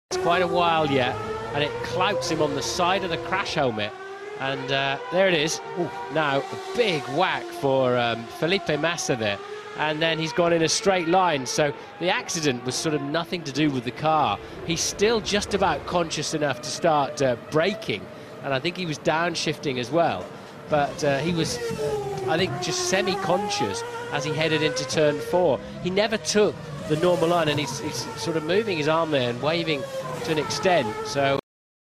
Formula 1 - Felipe Massa CRASH !!! HD HUNGARY 2009 (Budapest) - Race 10
Watch live race full length streaming for free only at: WatchFormula1.net.
FORMULA 1 MAGYAR NAGYDIJ 2009 (Budapest)
HUNGARY 2009 (Budapest)